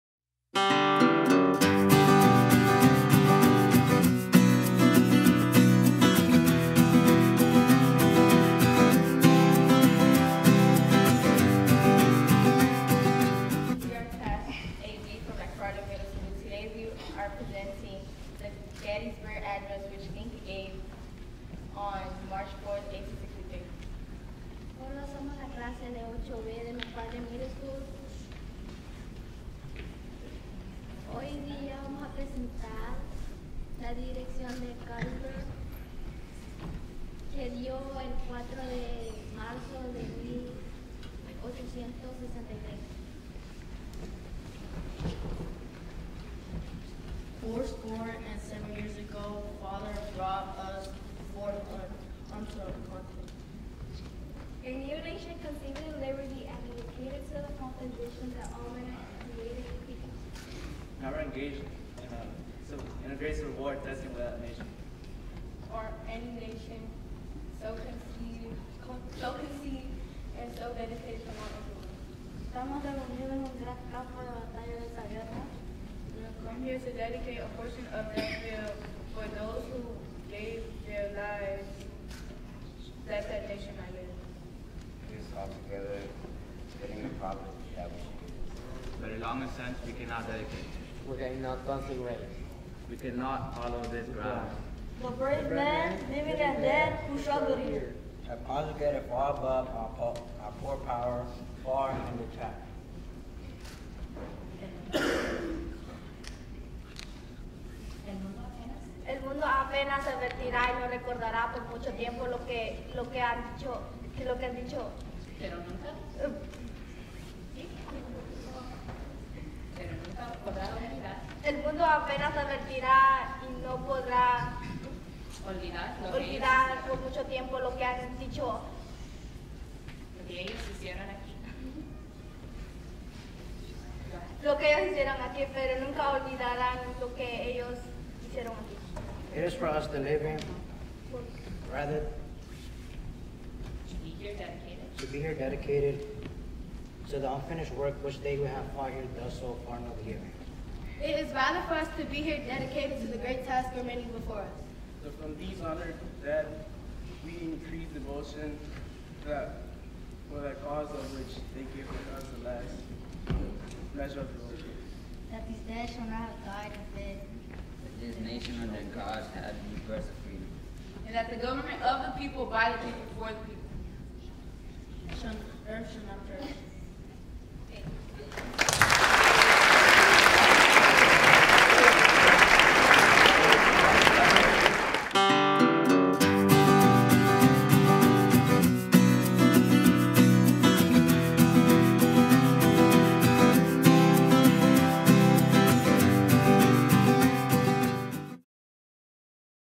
Ford’s Theatre | The Lincoln Oratory Festival 2019, MacFarland Middle School, D.C.
MacFarland Middle School, Washington, D.C., eighth grade
The Gettysburg Address (1863)
Classroom teacher Josue Otarola teaching artist Cecilia Cackley